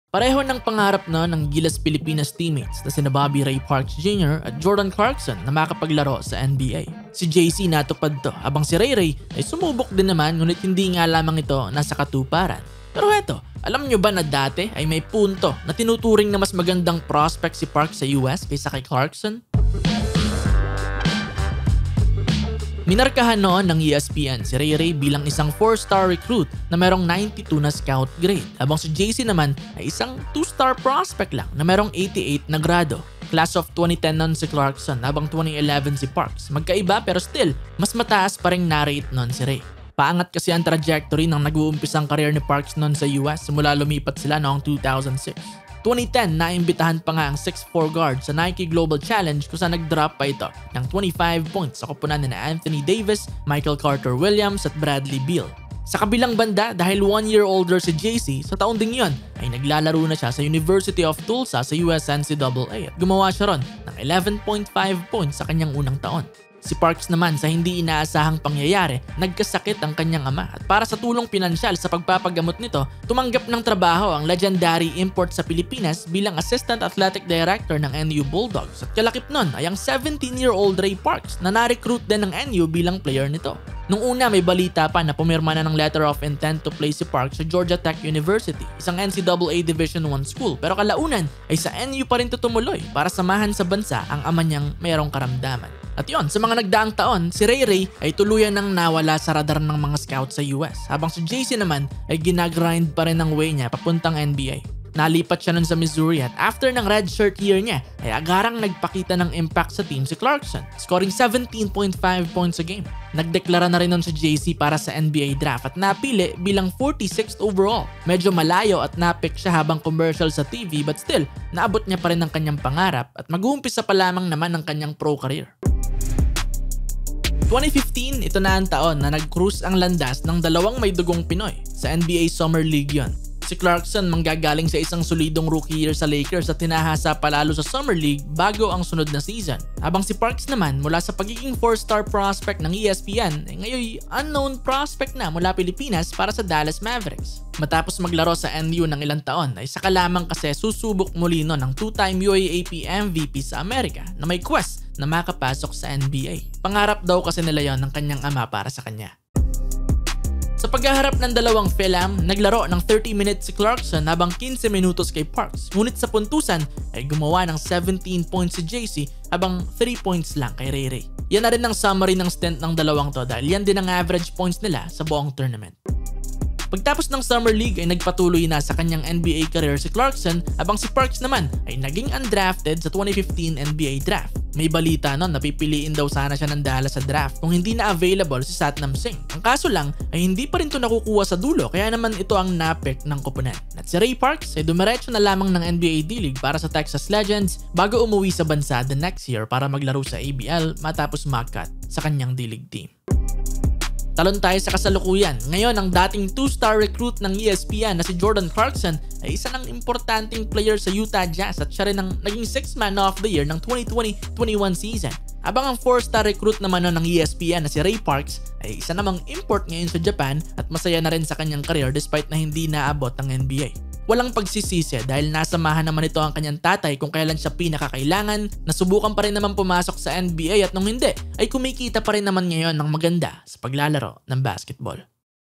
Noong Mas Bigating Prospect pa si Ray Parks kay Clarkson sa US
Pareho nang pangarap noon ang Gilas Pilipinas teammates na sina Bobby Ray Parks Jr. at Jordan Clarkson na makapaglaro sa NBA.

Si JC natupad ito habang si Rayray ay sumubok din ngunit hindi nga lang ito nasakatuparan.

Pero heto, alam niyo ba na dati ay mas mataas pa ang Ratings ni Parks kay Clarkson sa Amerika?